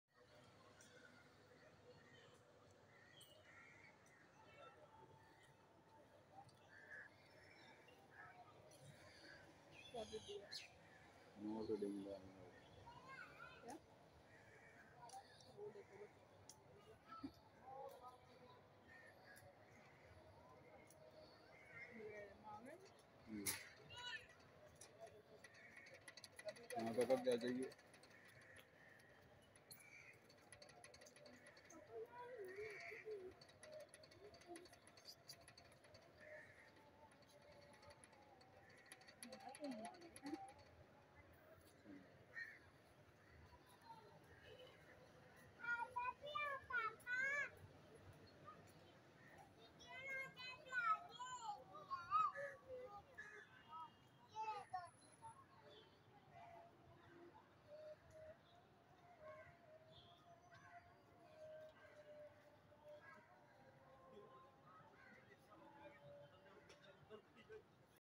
Axis Deer in the zoo. Axis deer is looking so beautiful and very lovely🥳 @Lovely birds and animals
this is called Axis Deer. I found this Deer in the zoo and I made this video for all Deer lovers. Axis deer is looking so beautiful and very lovely. deer is always my favorite animal and I love Deer very much. all animals wants love and care from us so we should be kind with them😊
@Lovely birds and animals